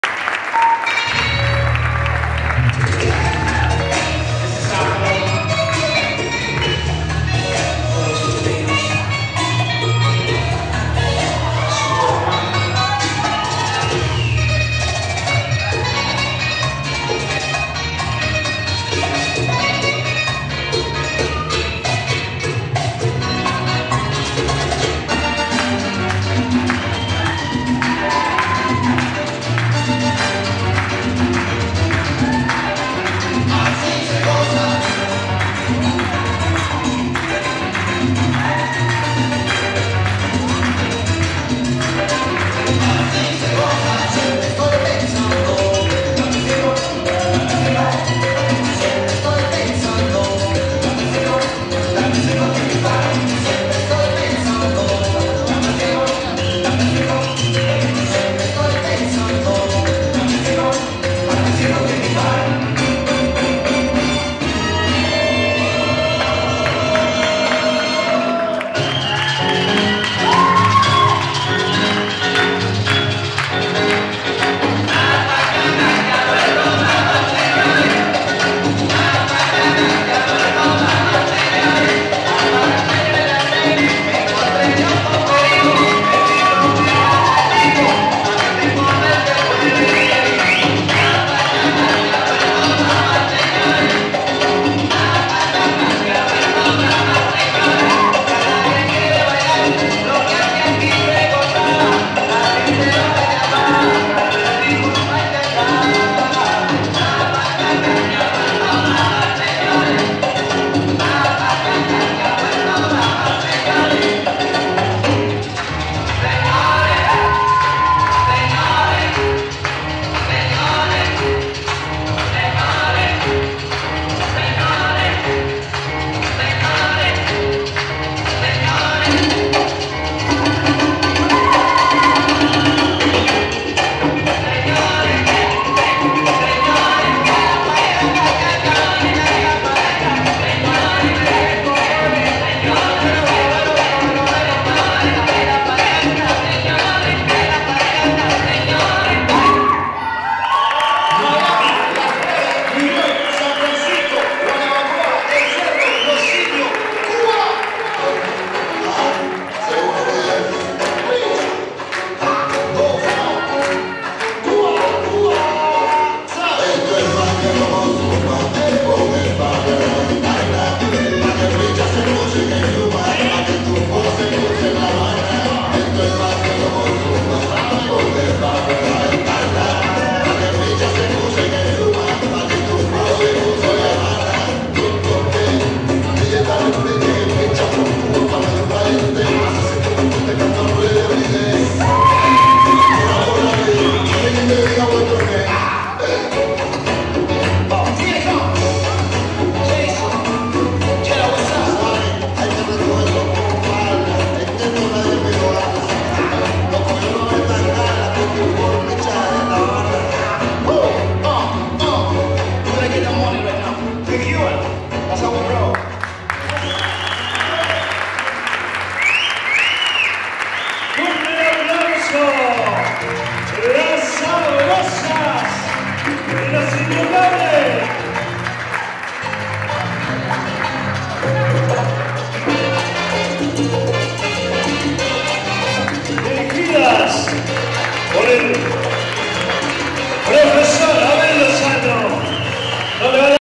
El Gran Fiestón 2014 ~ Sabrosas
Coreografía de estilo femenino presentada por el grupo Sabrosas, bajo la dirección de Abel Lozano. Evento organizado por Nelson Lara en la Sociedad Libanesa. Mendoza, Argentina. 31/10/2014